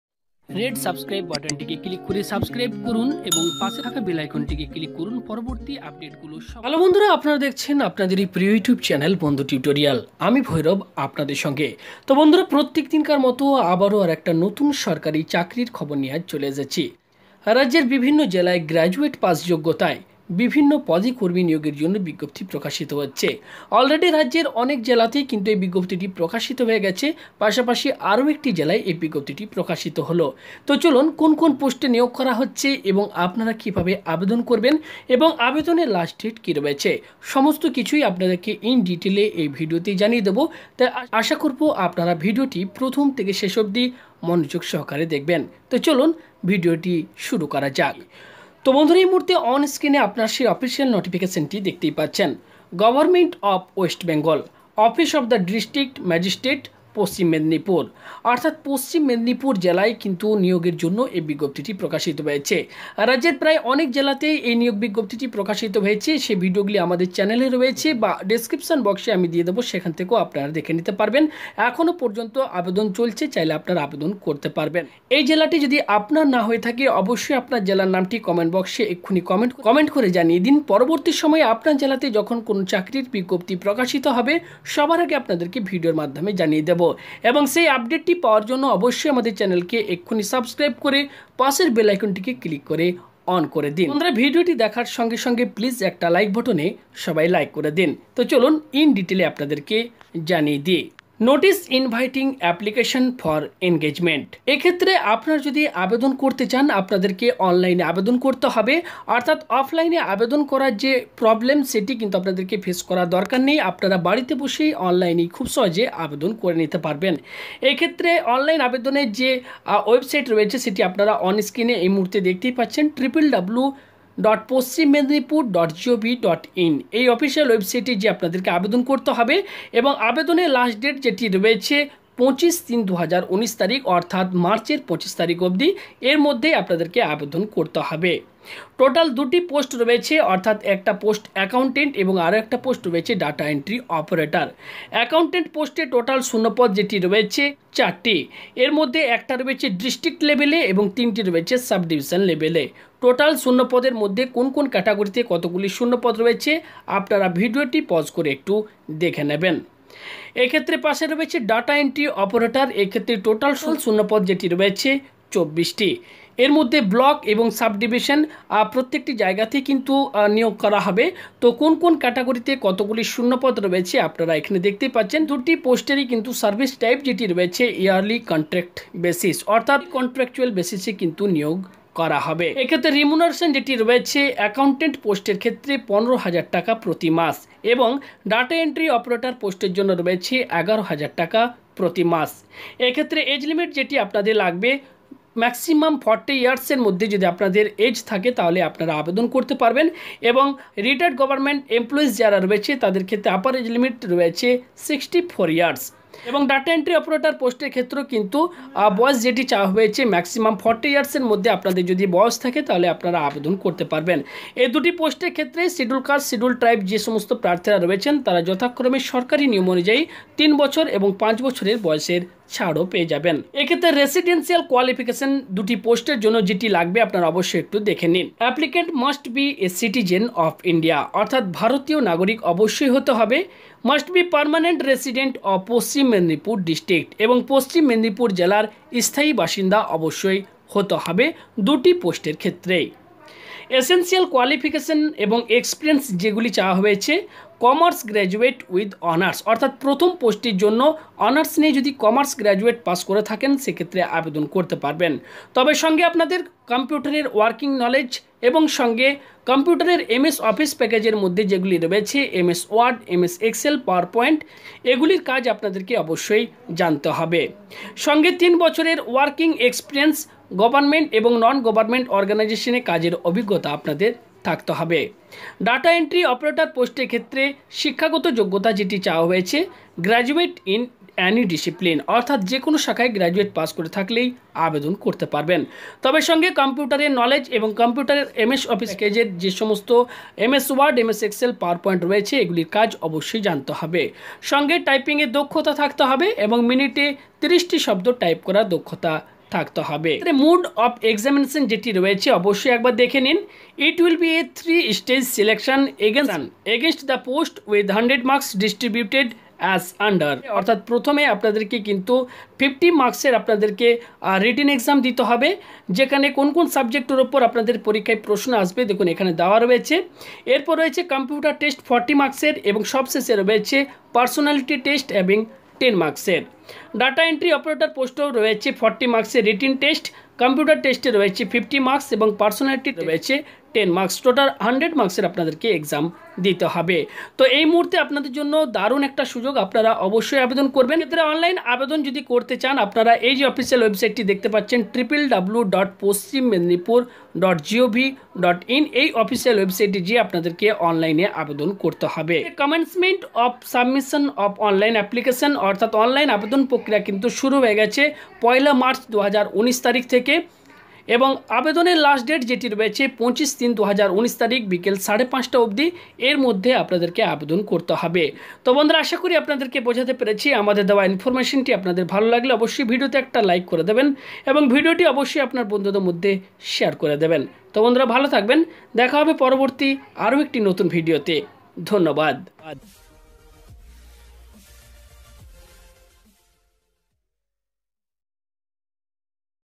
পশ্চিমবঙ্গে গ্র্যাজুয়েট পাসে প্রচুর কর্মী নিয়োগ | Data Entry Operator Recruitment 2019
পশ্চিমবঙ্গে গ্র্যাজুয়েট পাসে প্রচুর কর্মী নিয়োগের বিজ্ঞপ্তি | Data Entry Operator Recruitment 2019 | Wb Govt Jobs.